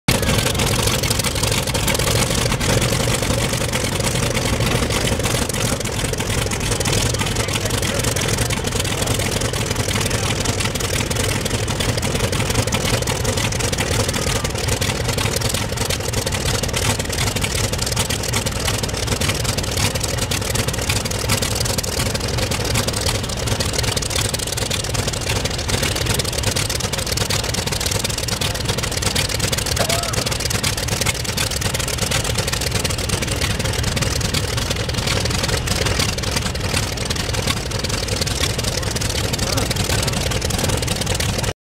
Focke Wulf 190 Start up.mov
Restored FW 190
Taken at Chino Planes of Fame Museum 1-8-2010